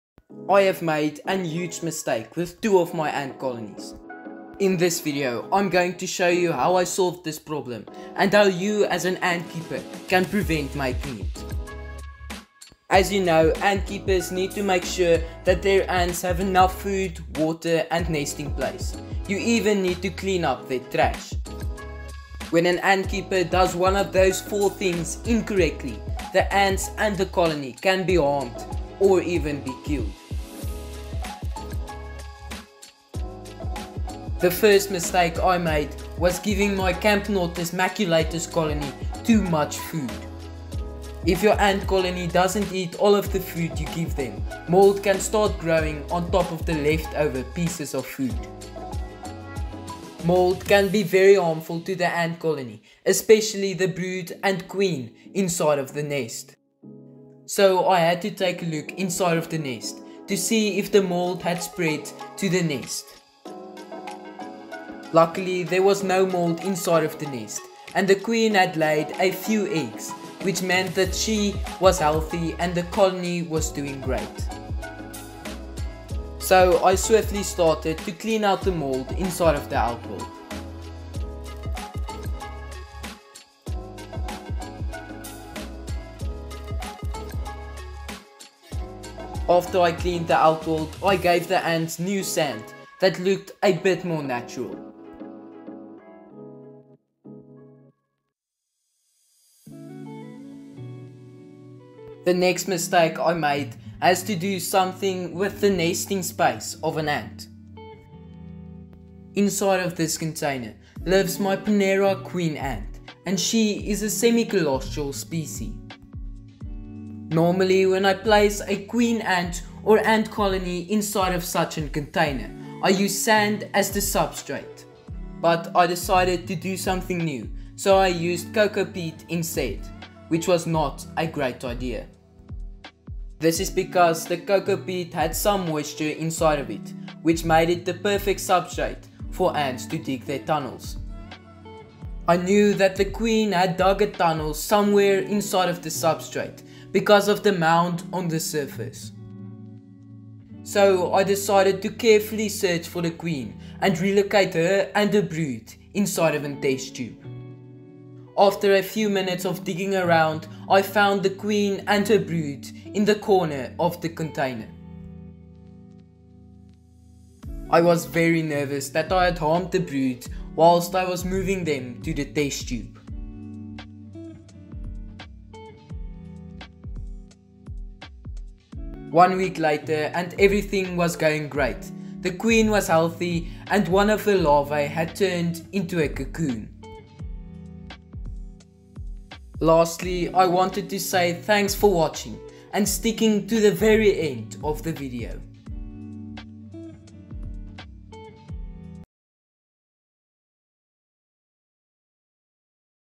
How To Fix Mold In An Ant Colony | Ponera Queen Ant
In this video I made 2 big mistakes with my camponotus maculatus colony and my ponera queen ant. I will also show you how I solved these problems

MR ANT
OUT